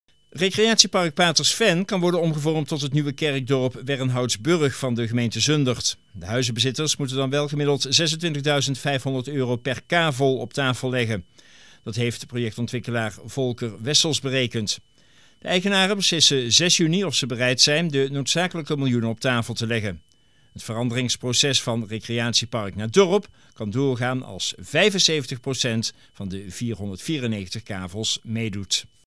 Patersven wordt zesde dorp van Zundert
Recreatiepark Patersven kan worden omgevormd tot het nieuwe kerkdorp Wernhoutsburg van de gemeente Zundert. De huizenbezitters moeten dan wel gemiddeld 26.500 euro per kavel op tafel leggen.

Bron: BN/DeStem-Brabant10